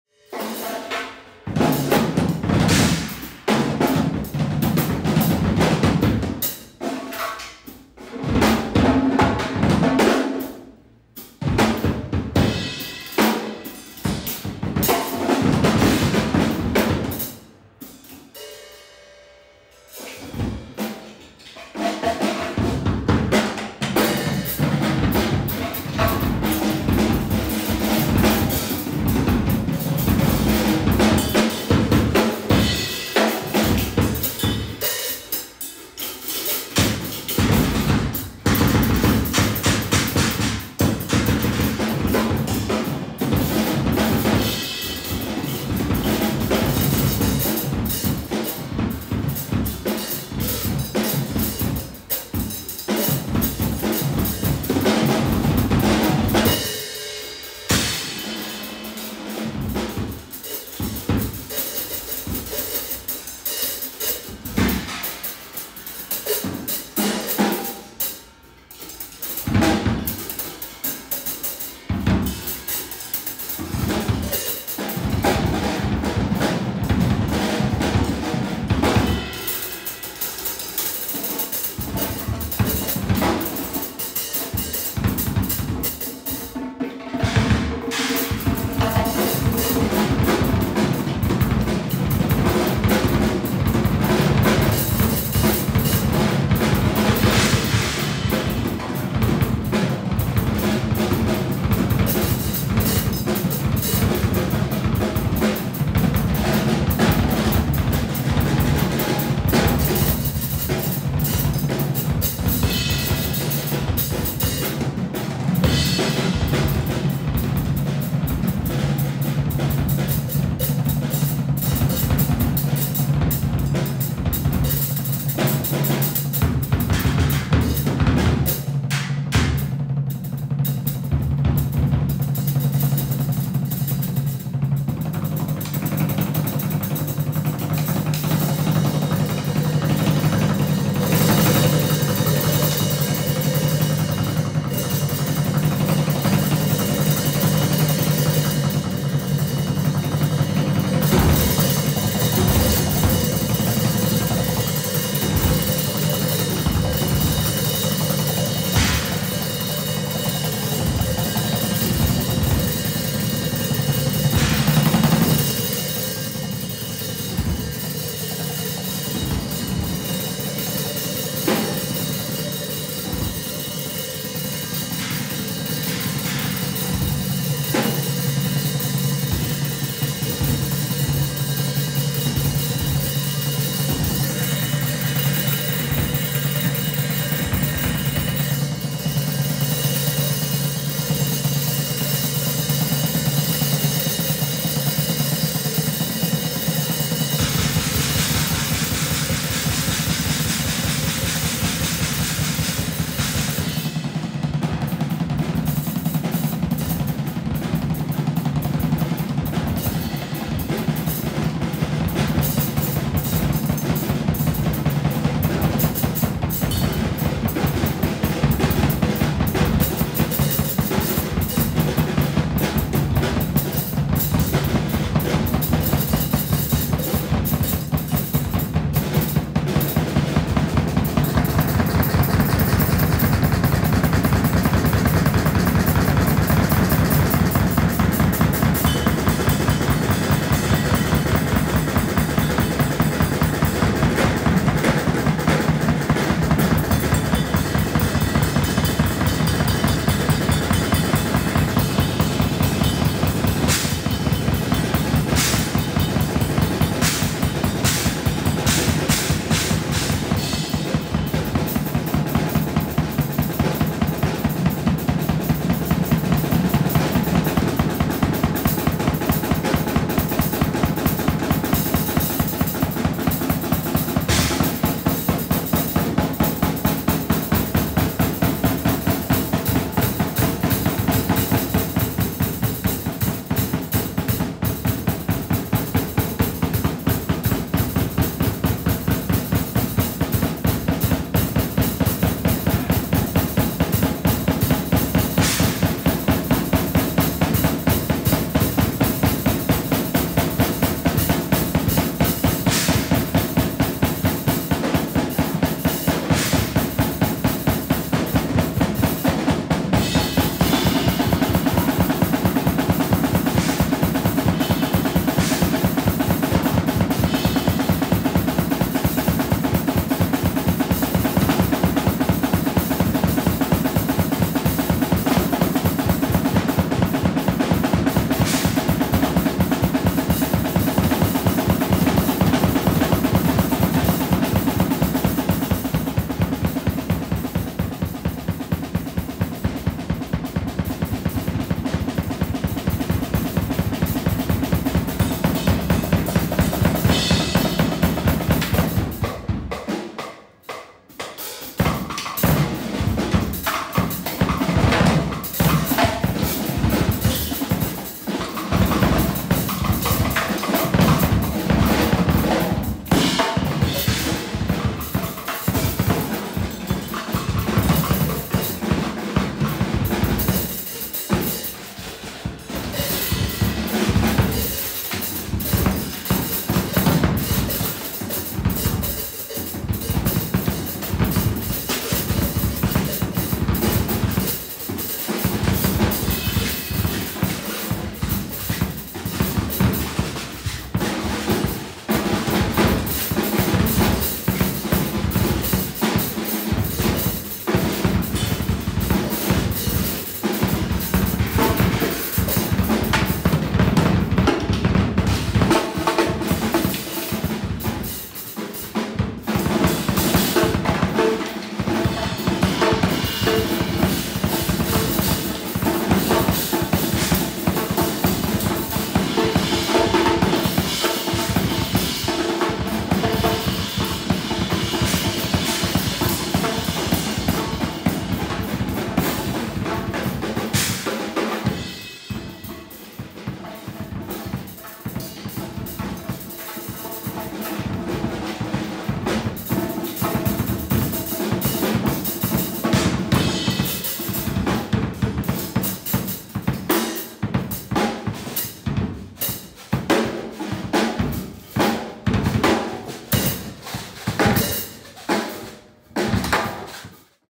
Watt Micks Duo
Watt Micks Duo in the Pink Room, The Crown, Baltimore MD January 17 2023